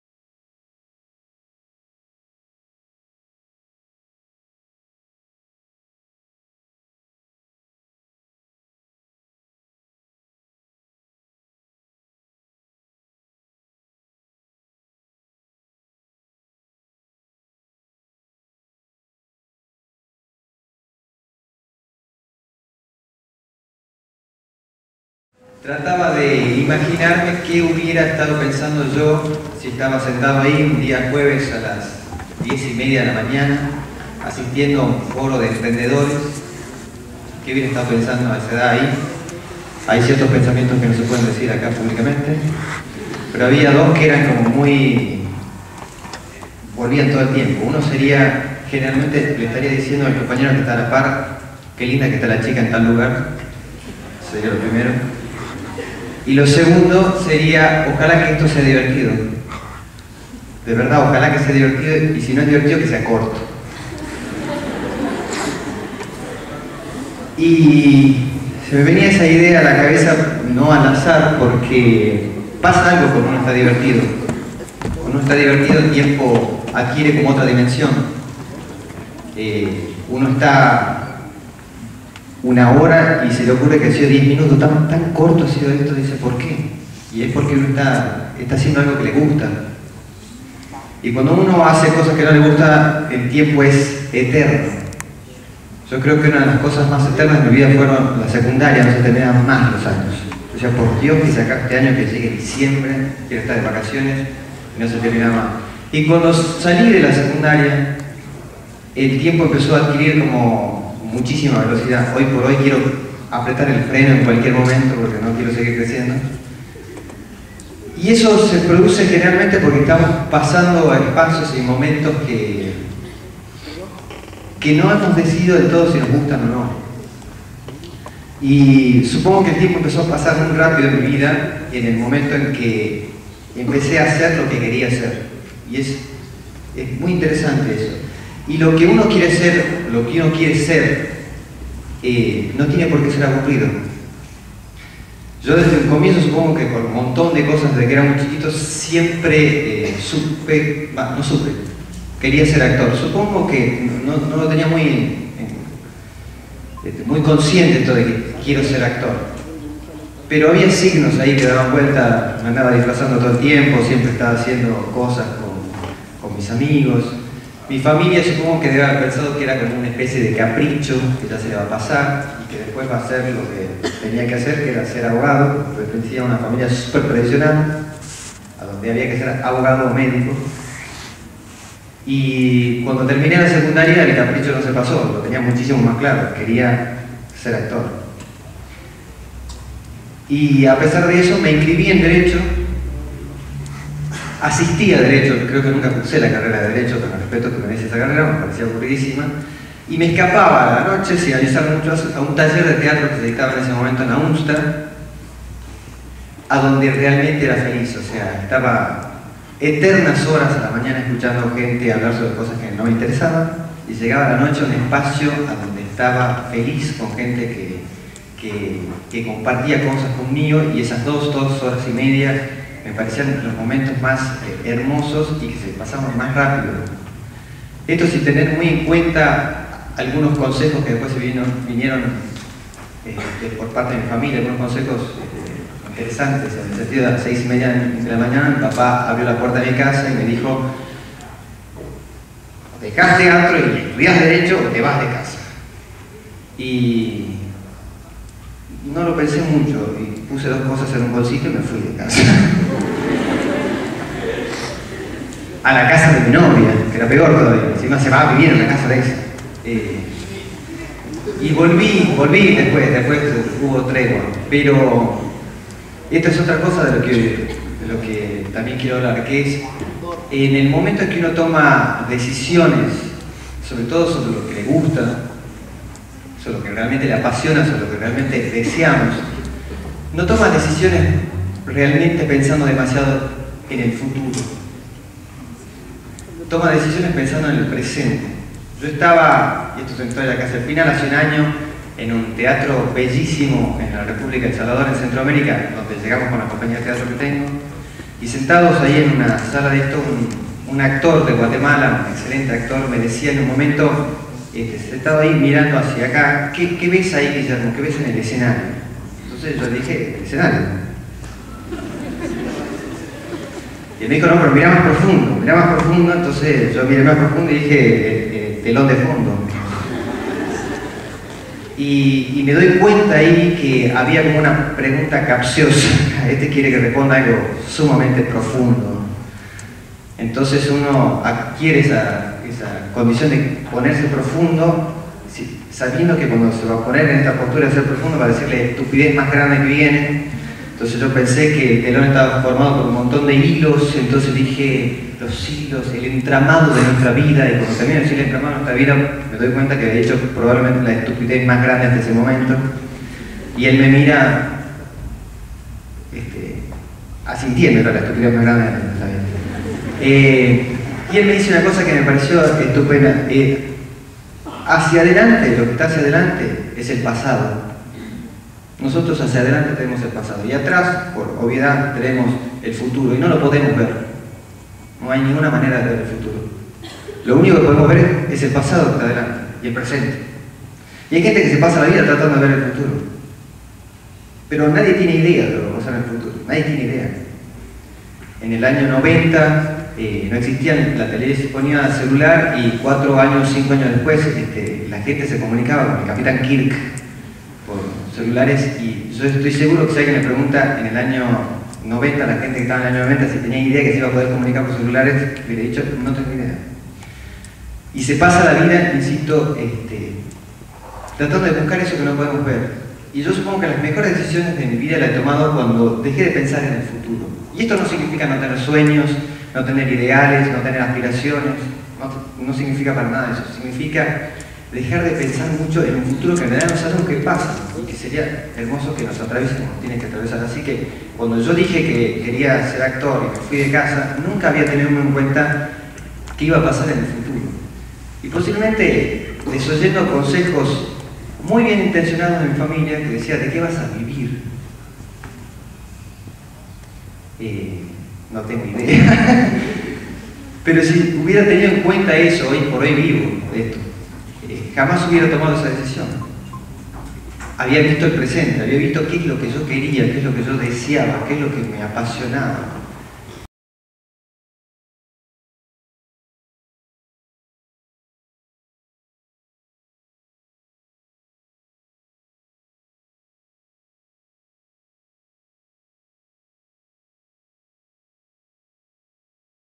Guillermo Montilla Santillán [Junior Achievement] Parte 1
Charla dada en octubre de 2011 para la Fundación Junior Achievement para jóvenes emprendedores